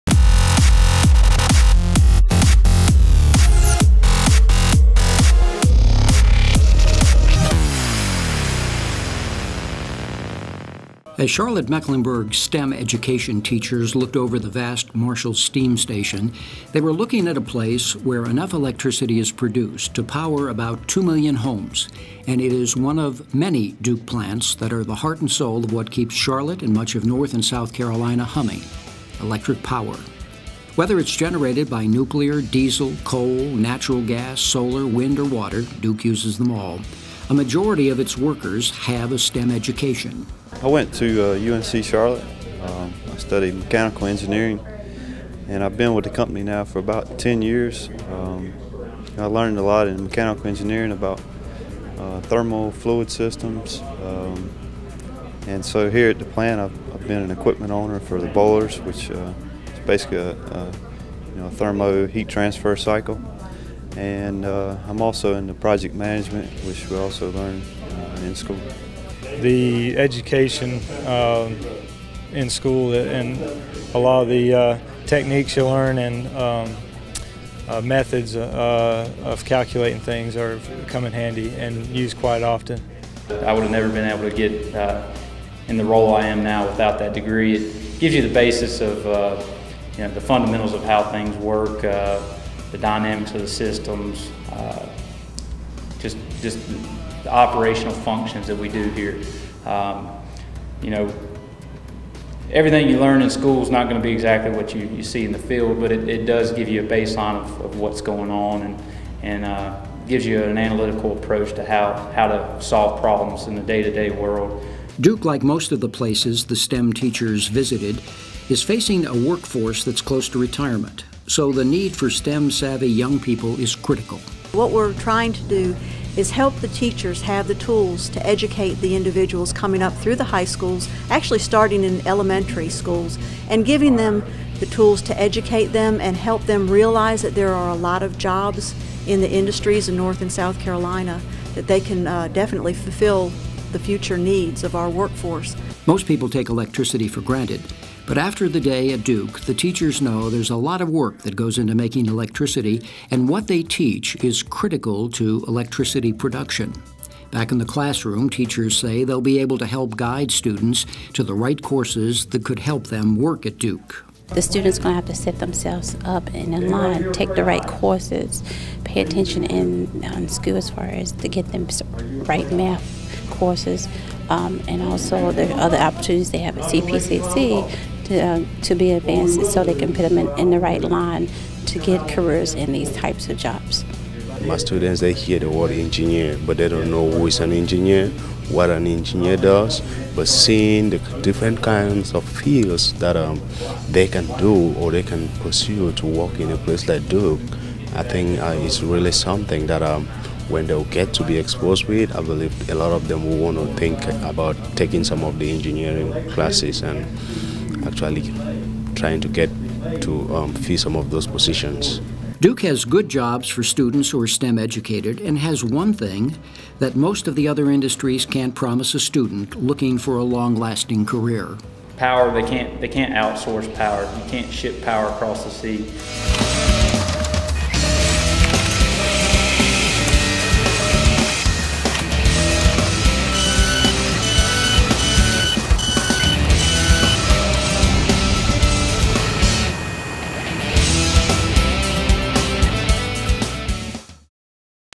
STEMersion2012 - Destination 3 - Duke Energy
Destination 3 for the CMS teachers participating in STEMersion2012 was Duke Energy, where the teachers got to tour the McGuire Nuclear Station and the Marshall Steam Station.

Over the next two weeks, we will be launching eight videos highlighting each destination for the STEMersion2012 event.  STEMersion is a partnership designed to educate teachers on STEM jobs currently in demand in the private sector.